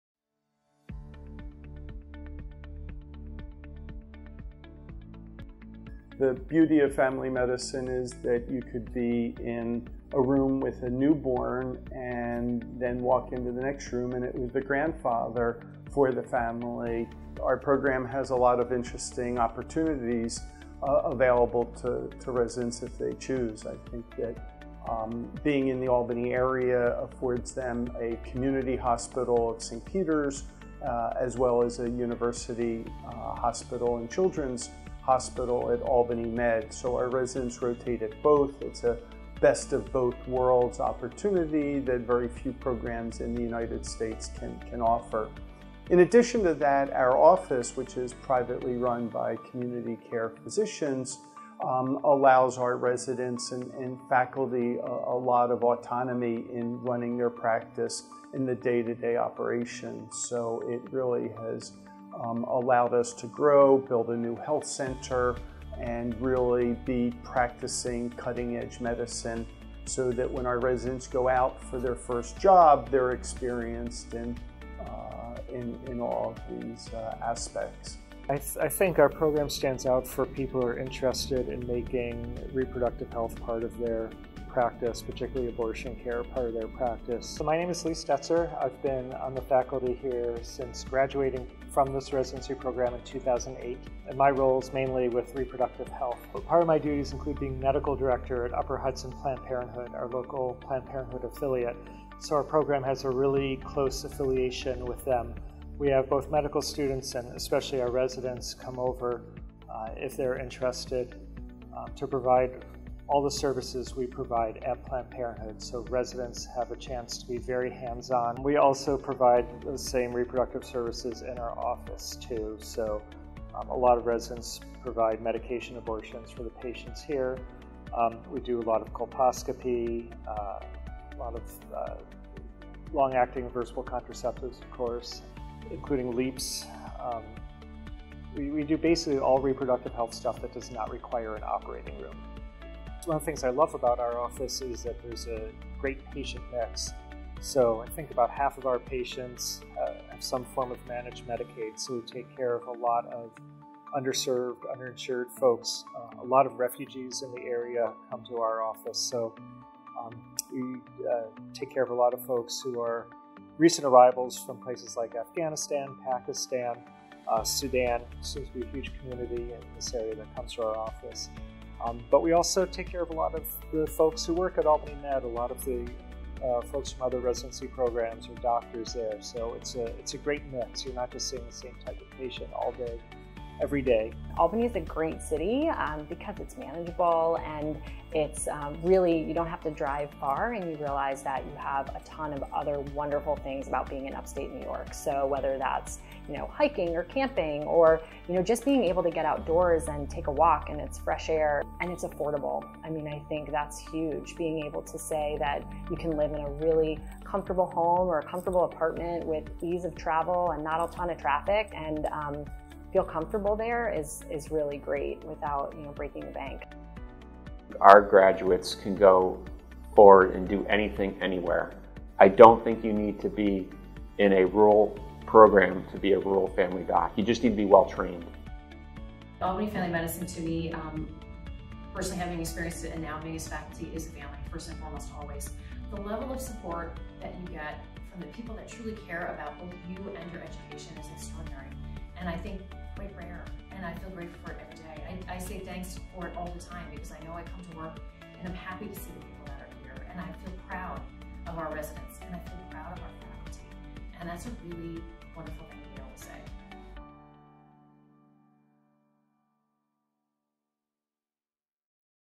REPRODUCTIVE HEALTH: Albany Family Medicine - AMC Residency Program
The family medicine residency of Albany Family Medicine and Albany Medical Center allows residents to determine which individual path they’d like to pursue. In this video, you'll learn about the program and its Reproductive Health path.